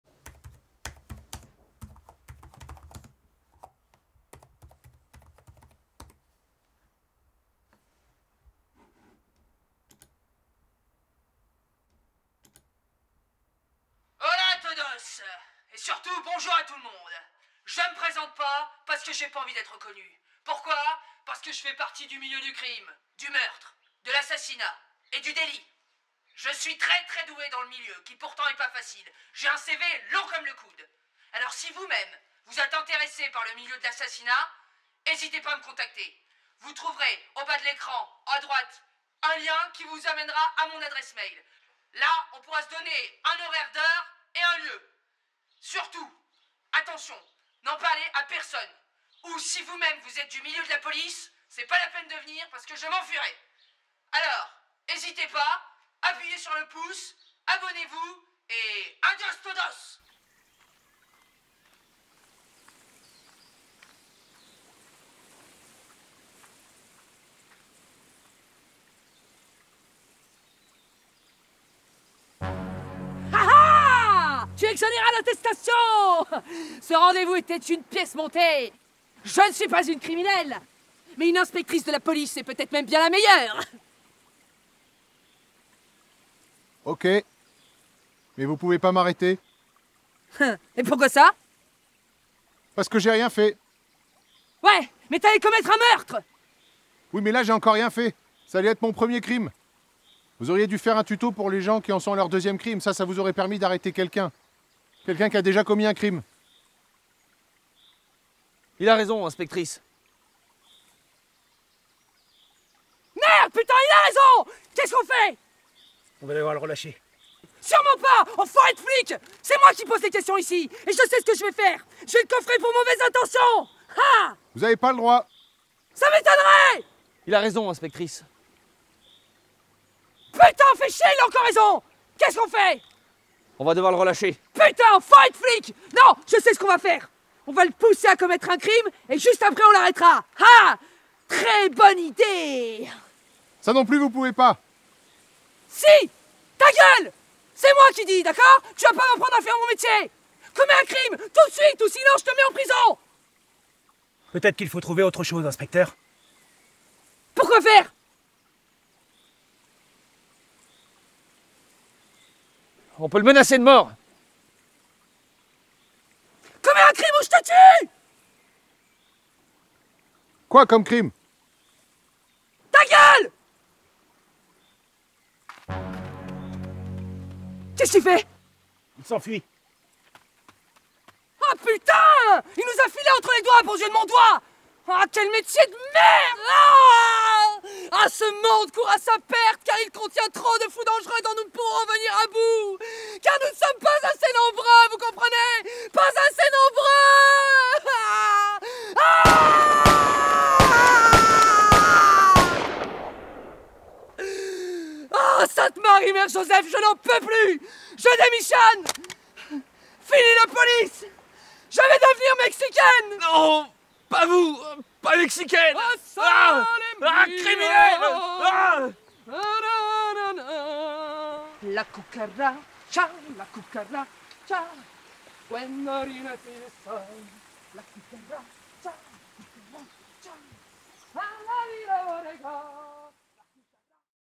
MARDI GRAS POLICE D'ÉTAT
Un crime est sur le point d'être commis. Notre brigade est sur le coup mais c'est chaud quand même.